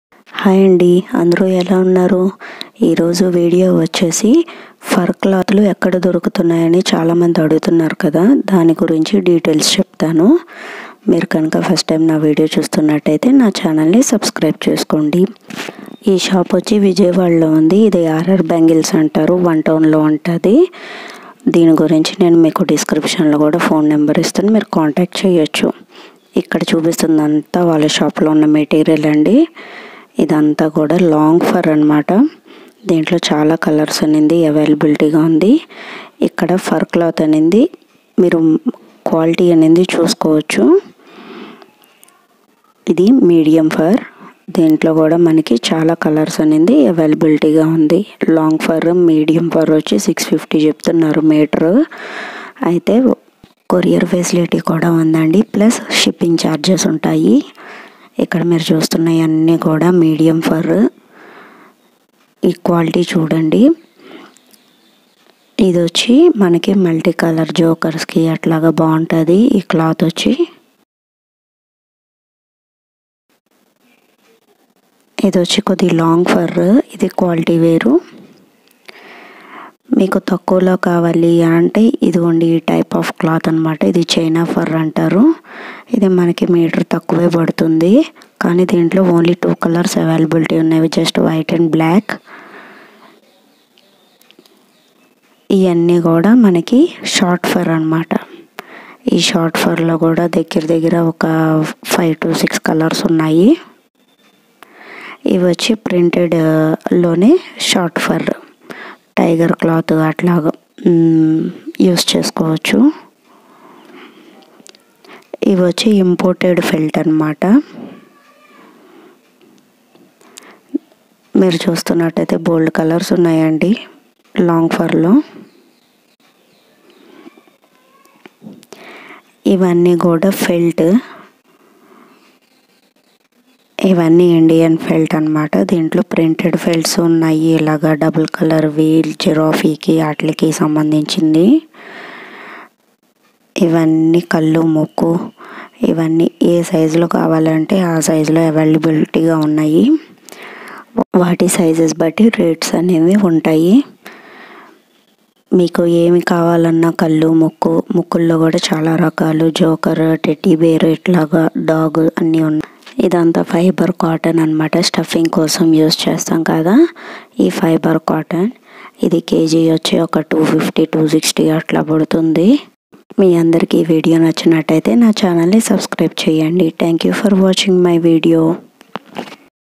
Soft toys fur cloth material shop / huge collection🛒
Today i am going to show the fur shop

Here there are huge variety of fur cloth materials for soft toys

Address
R R bangle
Vijayawada